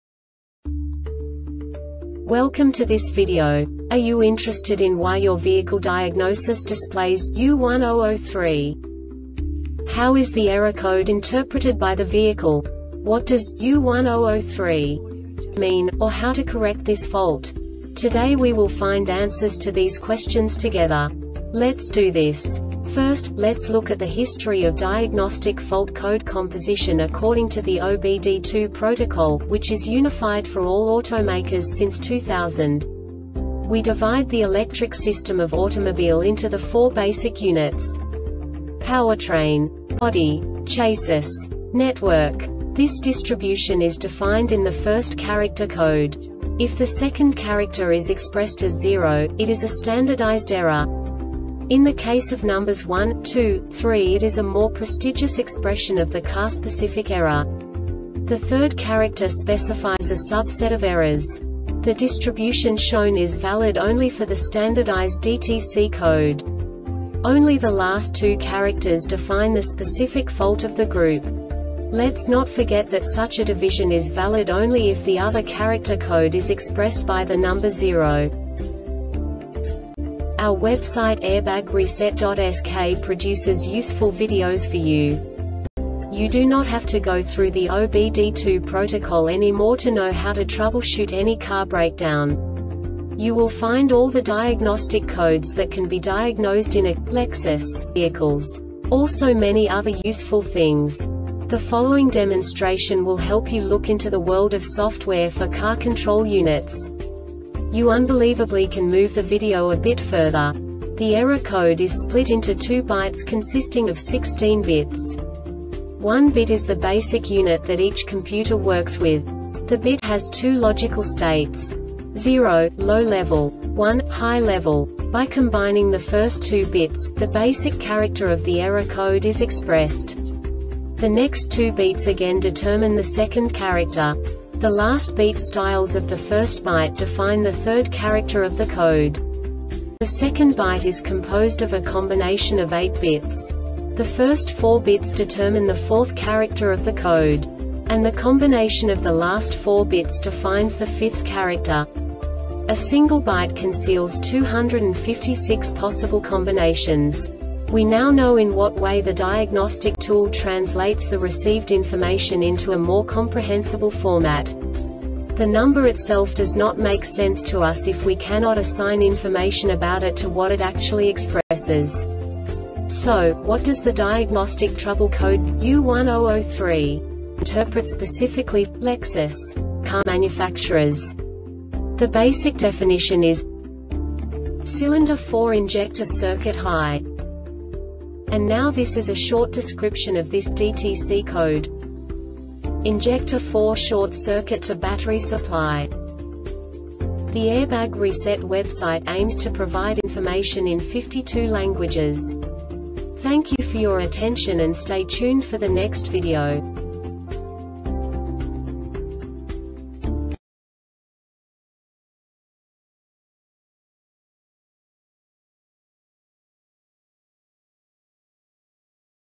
DTC Lexus U1003 Short Explanation
Contents:
0:21 Basic DTC analysis according to OBD2 protocol standard.
1:48 Insight into programming
2:58 A diagnostically understandable description of the Diagnostic Trouble Code
3:05 Basic error definition

Make:
Lexus
Code:
U1003
Definition:
Cylinder 4 injector circuit high
Description:
Injector 4 short circuit to battery supply